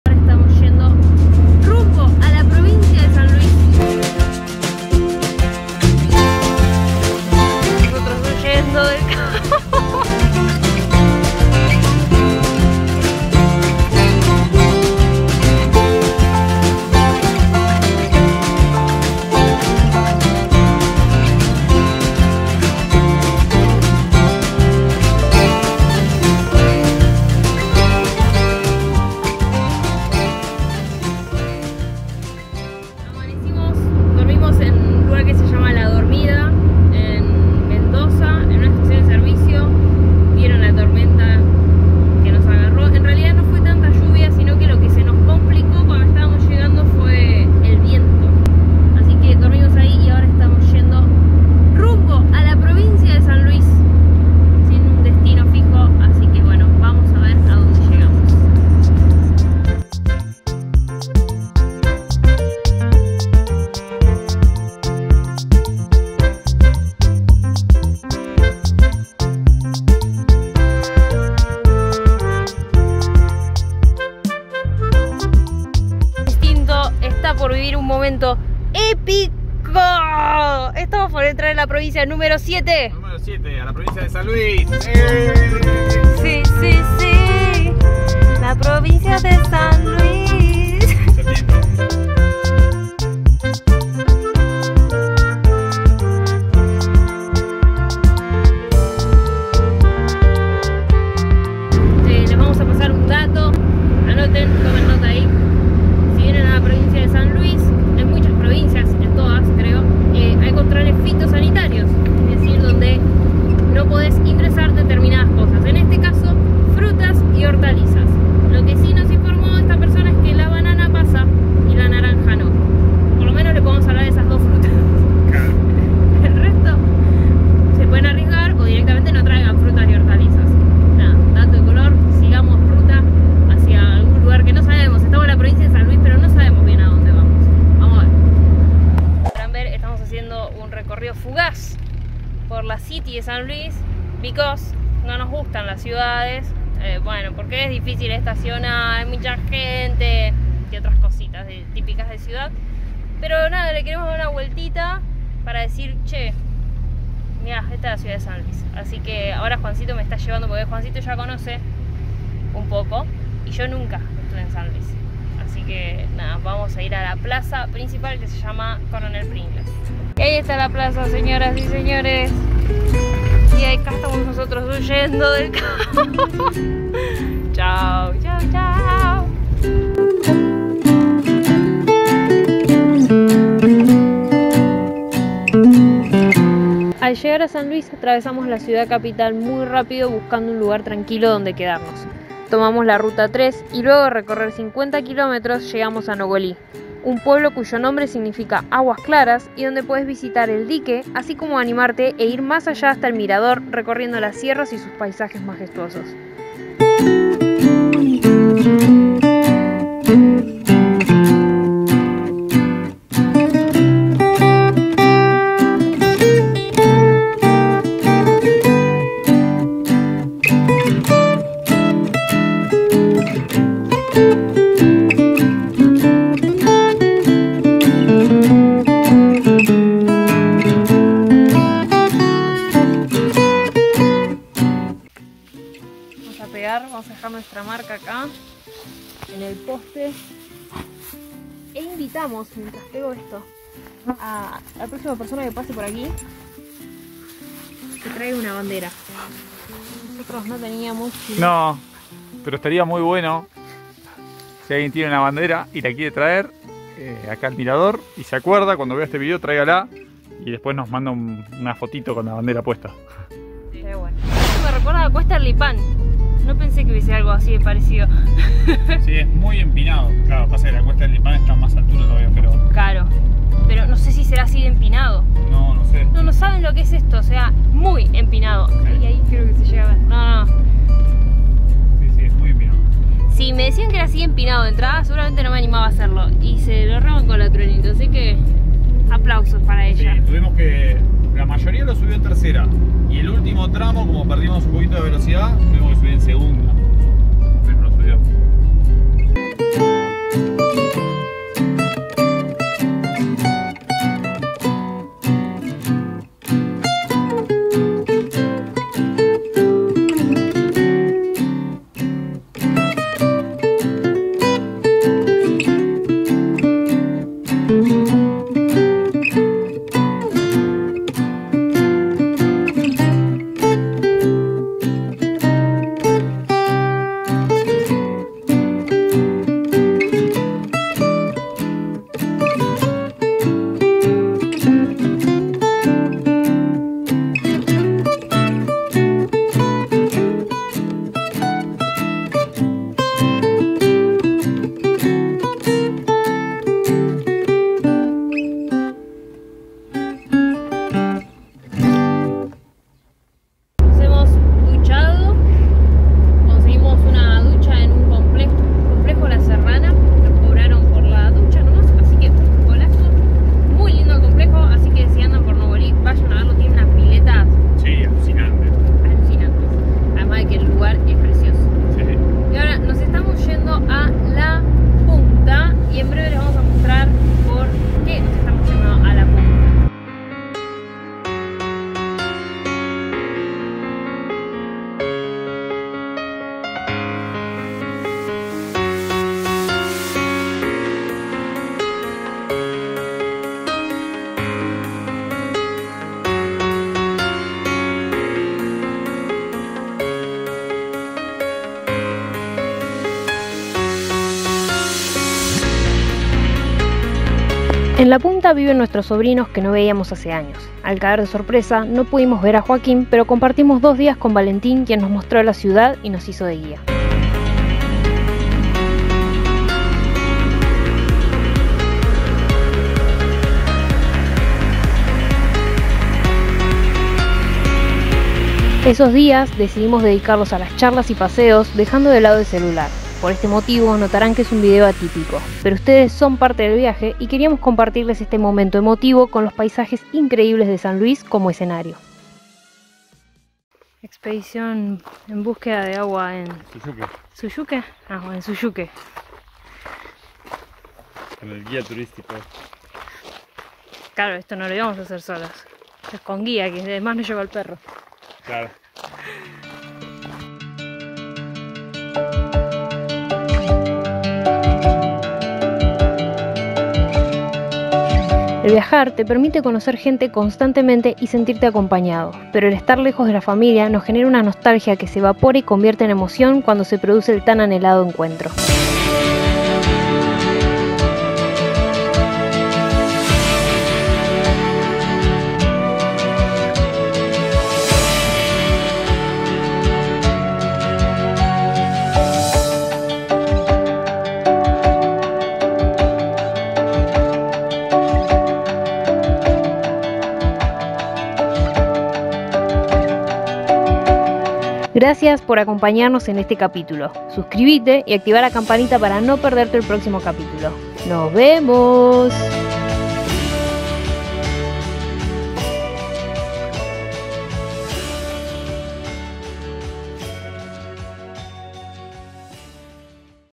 👉 Conocemos SAN LUIS de una manera DIFERENTE 【Recorriendo ARG en MOTORHOME】
Los invitamos a descubrir una nueva provincia juntos! En este espisodio recorremos La Punta, Nogoli y Suyuque Nuevo de una manera muy especial.
Los leemos en comentarios!!!